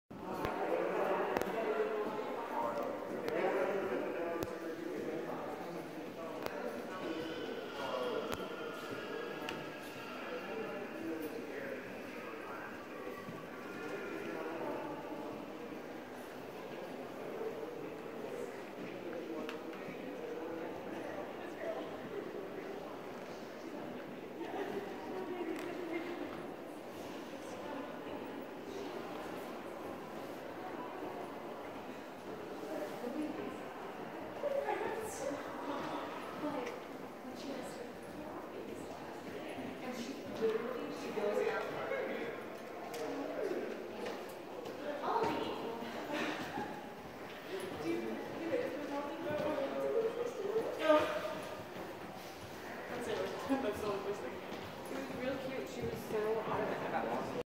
Joan Mitchell a Life Lived Through Color SFMOMA San Francisco, CA 20 December 2021 ART TOOLS
Joan Mitchell a Life Lived Through Color Exhibit at SFMOMA 20 December 2021 San Francisco, CA
A tour of Joan Mitchell's art supplies (art tools) as part of the exhibit.